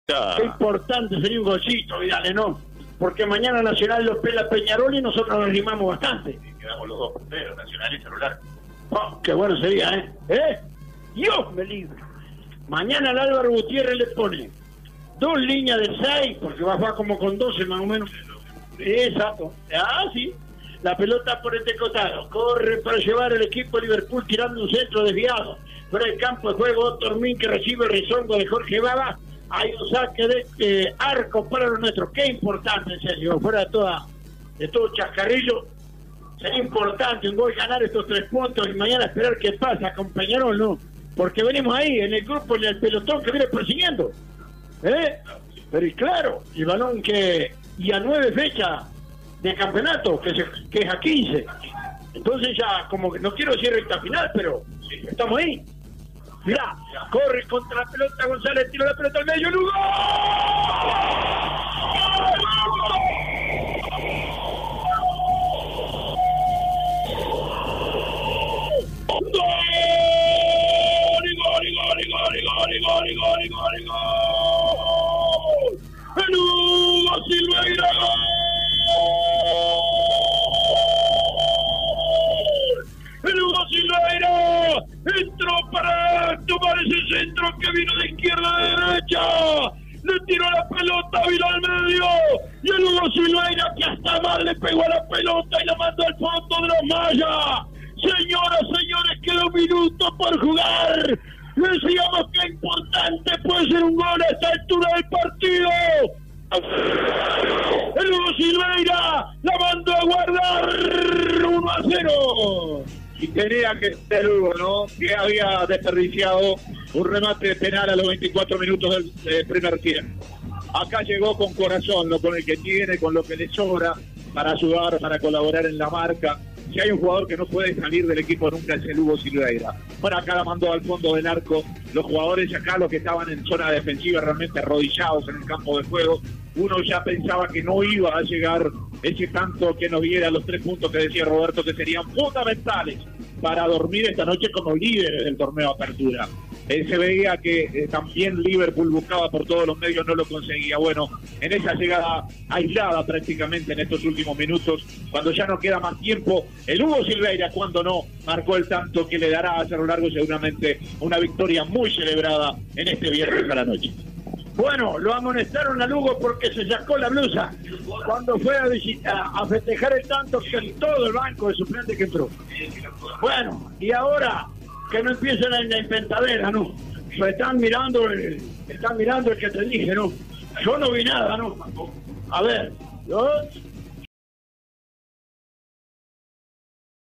Cerro Largo FC 1 Liverpool 0 - Apertura 2023 (por Roberto Silva)
Los goles del partido, por Roberto Silva y Gerardo David Vidales para CW 136 Radio Río Branco.